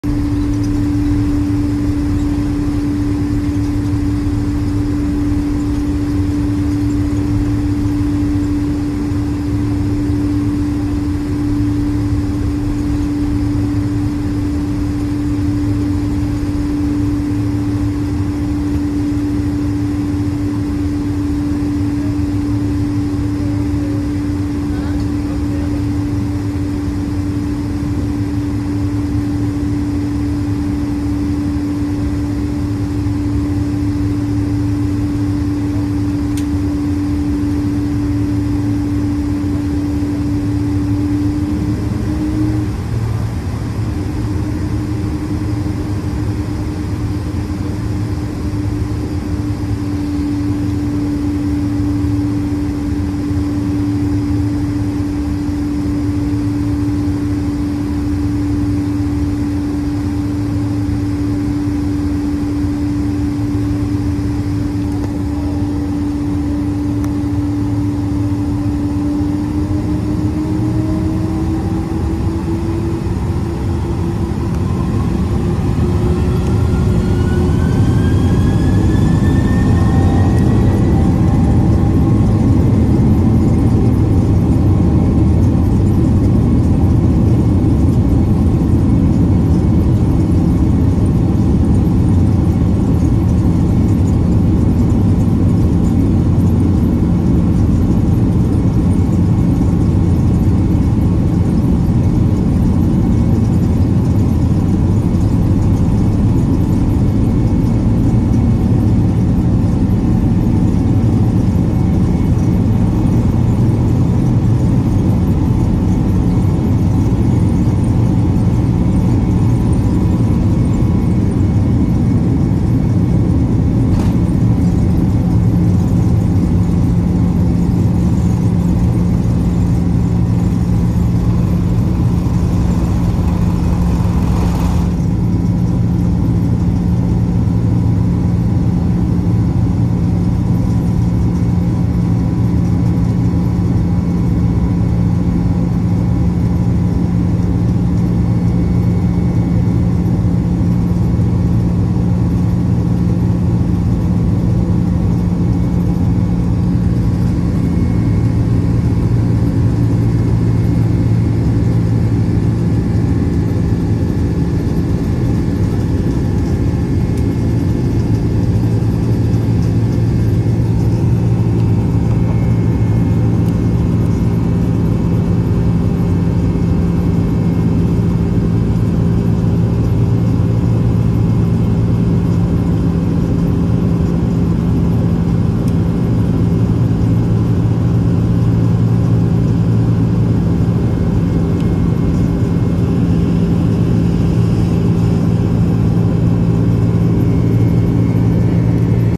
Takeoff from Honolulu | Delta 767-300
Takeoff from the reef runway of Honolulu airport, destination was JFK .
Delta Airlines , Boeing 767-300.